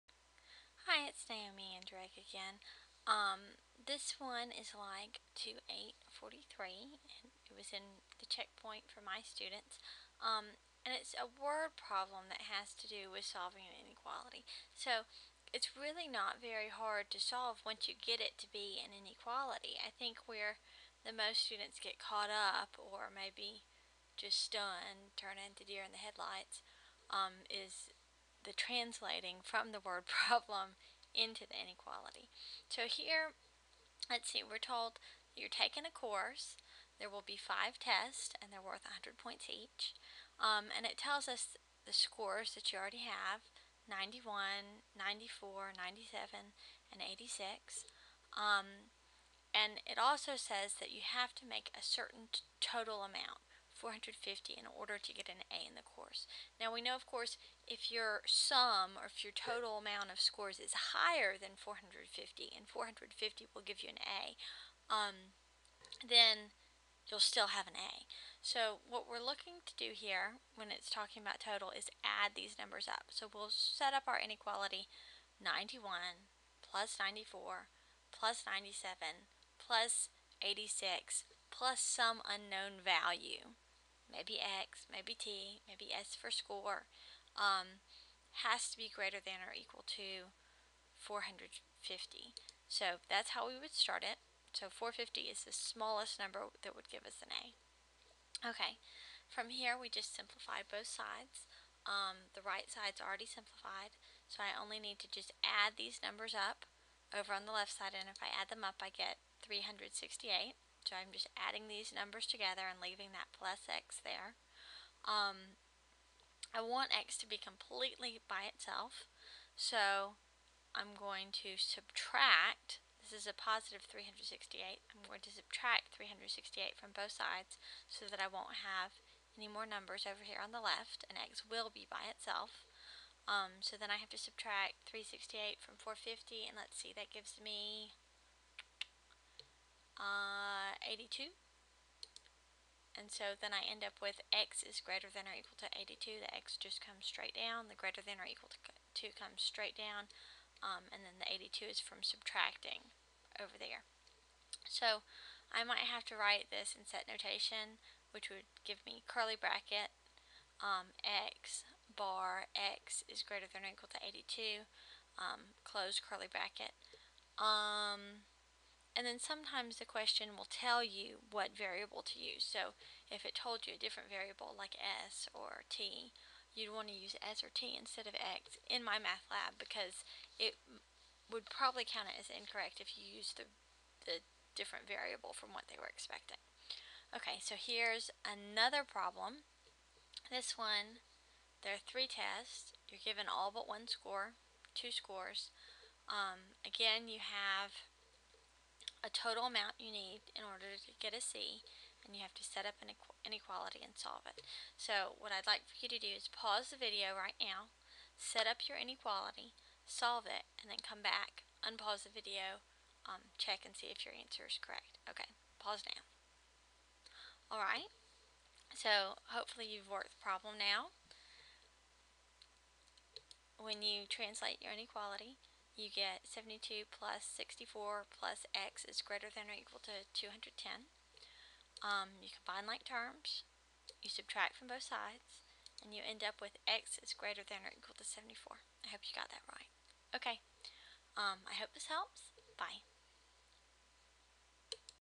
translating & solving inequality word problem
This video goes over translating and solving a word problem.